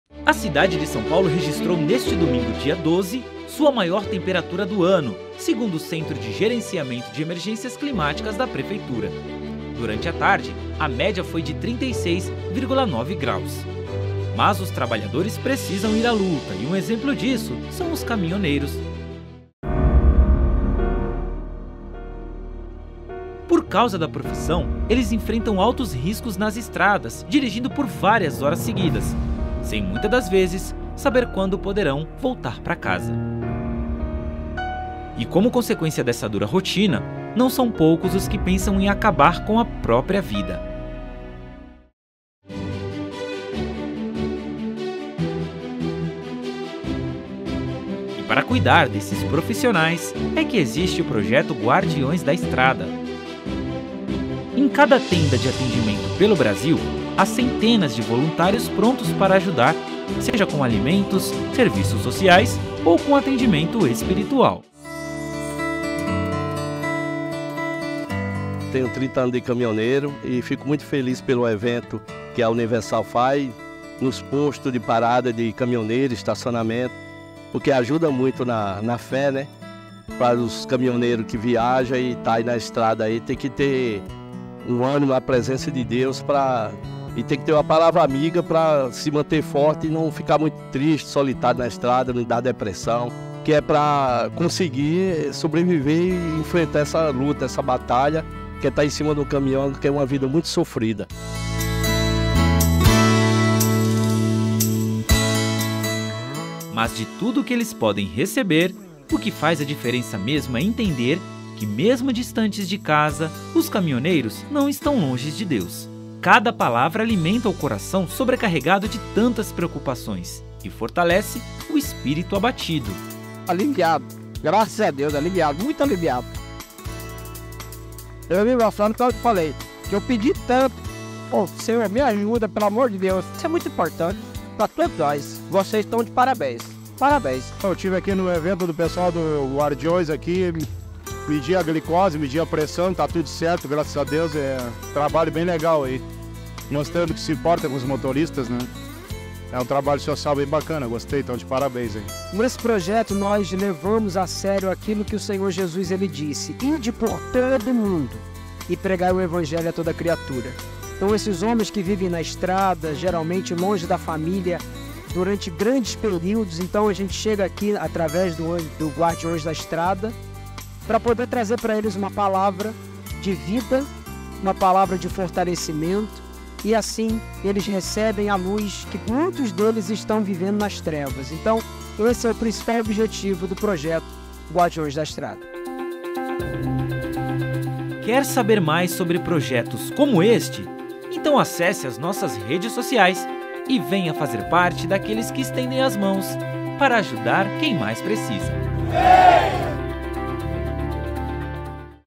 A RESPOSTA DE MUITAS ORAÇÕES
"Senhor me ajuda!"

Este é um grito de socorro, silencioso, mas que Deus ouve. Não importa o lugar, nem como foi feita a oração, Deus ouve e atende ao pedido sincero.

E isto é o que move o projeto Guardiões da Estrada: dar auxílio a aqueles que muitas vezes são esquecidos, os caminhoneiros.

Estes trabalhadores que sustentam e movem o país e que precisam também de sustento espiritual, para se lembrarem que não estão sozinhos, mas que Deus é com todo aquele que O invoca.